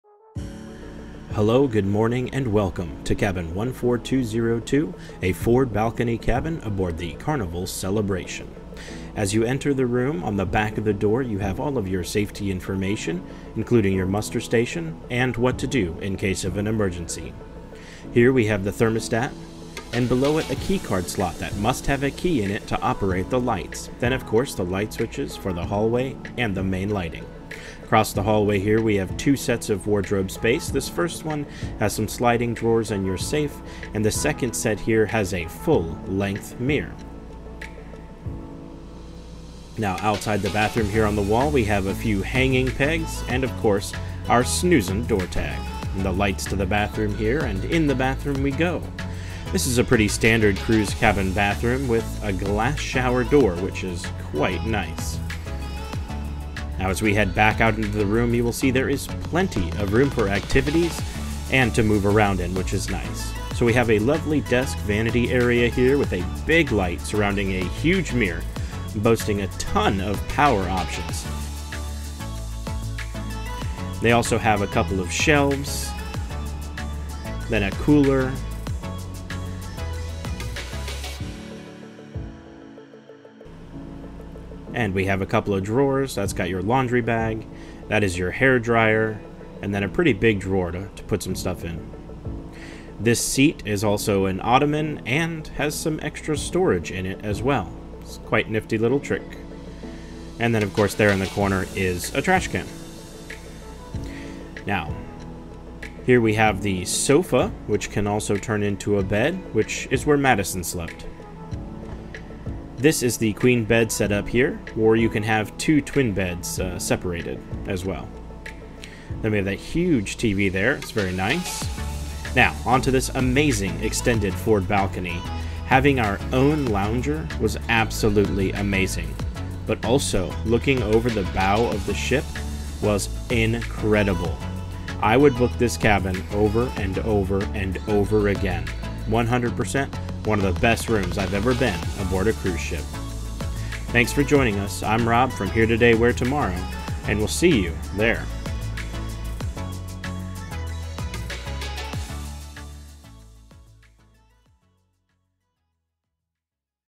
Carnival Celebration Forward Balcony Stateroom 14202 | WalkThrough
Come check out this Amazing Forward Facing Balcony Cabin Aboard the Carnival Celebration!
We have cruised many ships across a few lines and had many different types of staterooms. By far this was the best room we have ever had aboard a cruise ship! Check it out!